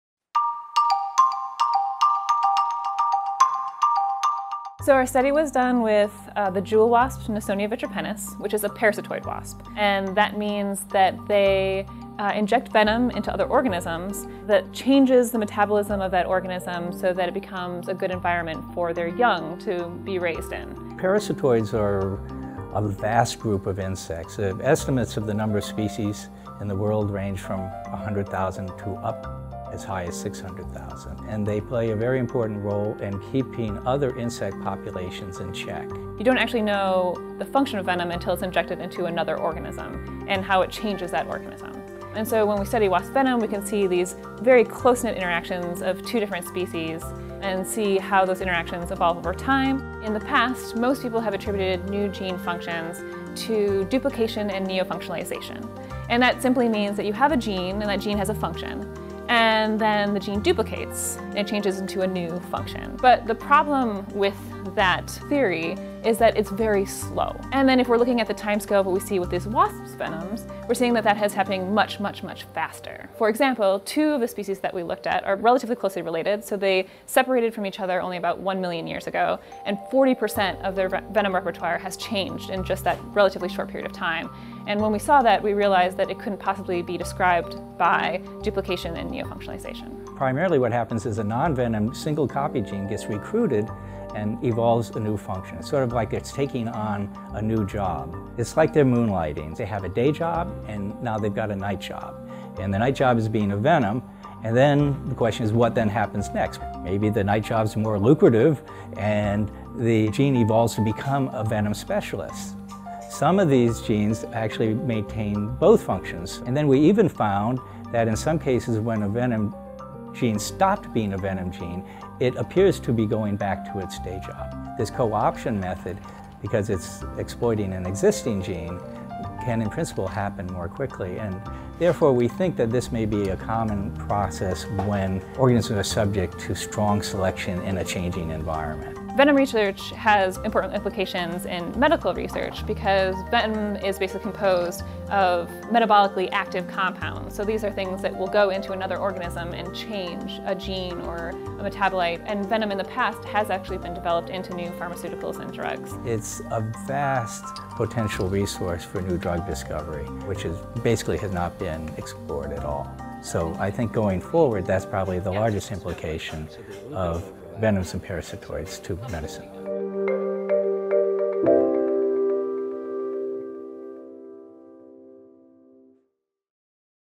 A Tiny Wasp, with a Big Evolutionary Secret to Tell
Amid the incredible diversity of living things on our planet, there is a common theme. Organisms need to acquire new genes, or change the functions of existing genes, in order to adapt and survive.

How does that happen?

A common view is that genes duplicate, with one of the copies picking up a new function while the other copy continues to function as before.

However, by studying tiny parasitic Jewel Wasps and their rapidly changing venom repertoires, the Werren Lab at the University of Rochester has uncovered a different process that may be widespread in other species as well.

The process involves co-opting single copy genes to take on new functions. In some cases, these genes appear to continue their previous function as well, in other parts of the wasp’s anatomy besides the venom gland.  The findings are published in Current Biology.

“It is almost as if they are now moonlighting,” says John (Jack) Werren, professor of biology. “They’ve got a day job, and then take on a night job as well. Over time, if the night job works out, they may give up the day job and evolve as a venom specialist.  However, in other cases we have found that they stop moonlighting as venom genes but appear to retain their day job.”

How is a gene co-opted? And what determines which job (or combination of jobs) it performs?  In the case of Jewel Wasps, the process called gene regulation is key. As the researchers explain, the rapid turnover in venom genes is accomplished mostly by changes in regulatory regions adjacent to the genes. These regulatory regions control how the genes are expressed—that is, whether the genes are turned “on” or “off” in different tissues. When a gene is turned on, it provides instructions for manufacturing proteins. When it’s turned off, it provides no such instructions.

"Co-option of single copy genes can be a more rapid mechanism for adapting to a new environment because it does not require the gene to be duplicated first," notes Ellen Martinson, a co-lead author and postdoctoral research associate in the Werren Lab.

The group—which also includes co-lead Mrinalini, a former postdoc in the Werren Lab who has since joined the National University of Singapore, and two other researchers at the University of Rochester, Yogeshwar Kelkar and Ching-Ho Chang—studied four closely related species of Jewel Wasps. The wasps lay their eggs on the pupae of other insects, after first injecting the pupae with venoms that manipulate the metabolism of the host in ways that make the environment more conducive to their developing young.

Using transcriptome and proteome sequencing, the researchers found that more than half of the venom components in the parasitic wasps resulted from single copy genes that had been co-opted without being duplicated. The composition of the venoms can change quickly, allowing the wasps to adapt to different hosts. For example, even closely related species can differ by up to 40 percent of their venom repertoire.

The group proposes that co-option of single copy genes for new function is not just restricted to parasitoid venoms.  Co-option may be common in nature, particularly when organisms are evolving rapidly to changing environments.

With regards to parasitoid venoms, there may be an added benefit, says Werren. "The great diversity of parasitoid venoms and abundance of these species (estimates run as high as 600,000 parasitoid species on earth), combined with the fact that parasitoid venoms have evolved to manipulate metabolic processes, suggests that they are potentially an immense untapped cornucopia for drug discovery."